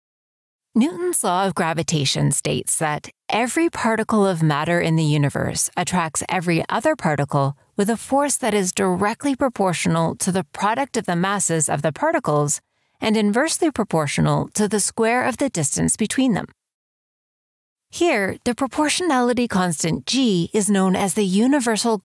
Newton's Law of Gravitation | Physics | Video Textbooks - Preview
Newton's law of gravitation states that "Every particle of matter in the universe attracts every other particle with a force that is directly proportional to the product of the masses of the particles and inversely proportional to the square of the distance between them."
 Here, the proportionality constant G is known as the universal gravitational constant. Its value is 6.